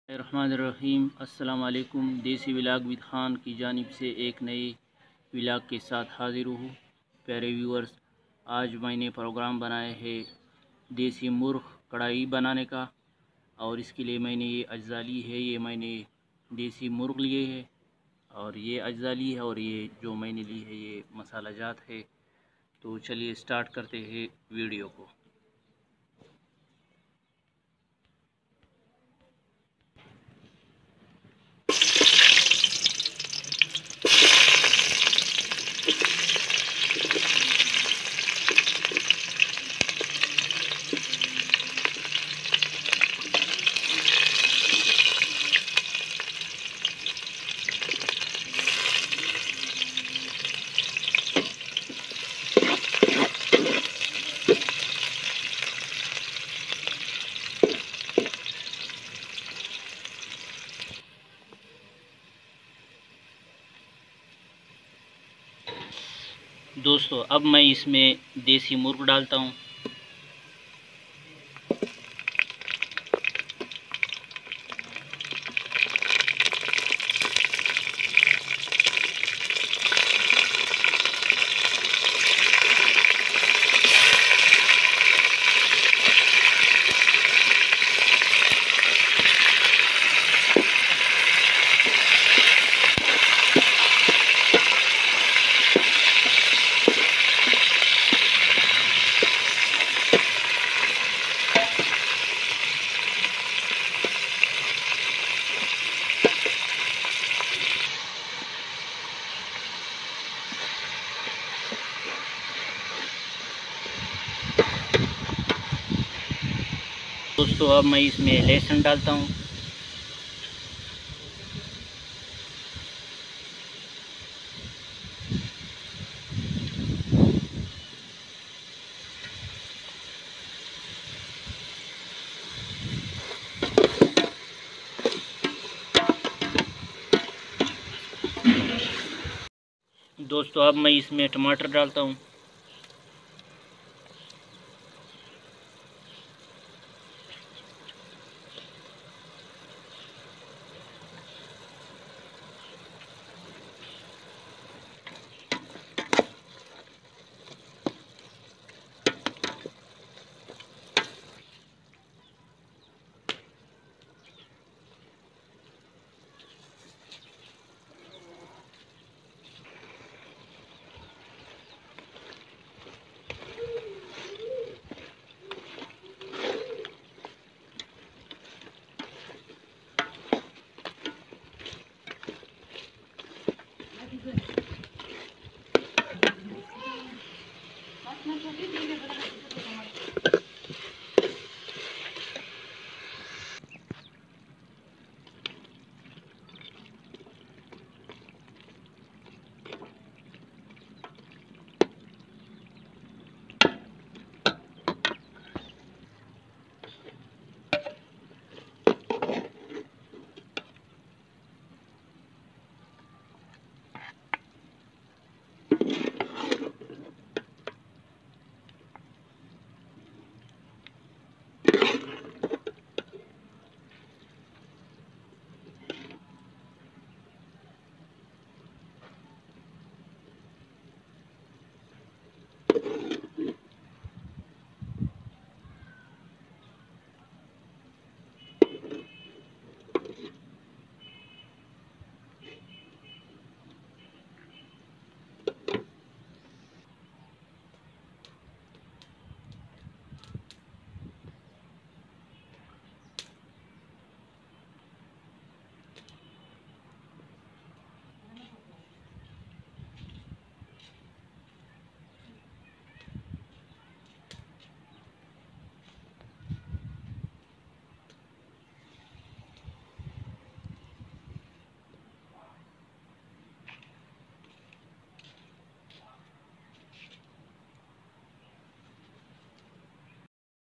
Desi Murgh Karahi Recipe //desi chicken karahi//desi vlogs with khan
in this vlog i will show you how to make desi murgh karahi recipe